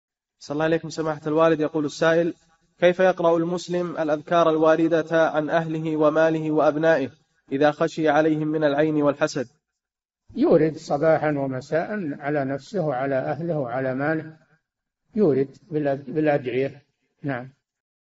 [353- 4717] كيفية الرقية على الأهل والأولاد - الشيخ صالح الفوزان
"[4717-353] كيف يقرأ المسلم الأذكار الواردة عن أهله وماله وأبنائه إذا خشي عليهم من العين والحسد؟
الجَوَابُ: يورد صباحًا ومساء على نفسه وعلى أهله وعلى ماله يورد بالأدعية.

للاشتراك في قناة الشيخ صالح الفوزان
فتاوى المجالس في تفسير المفصل

للاستفسار والإقتراحات والدعم
رئيس الفريق
لمشروع كبار العلماء